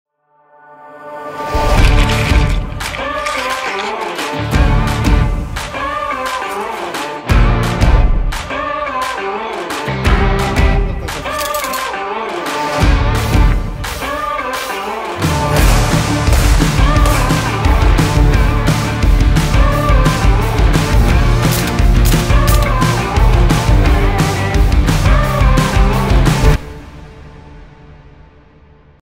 G. Gerlach visits shooting range to play big boys toys :-)
Starring The Pilot and the Bathyscaphe models :-)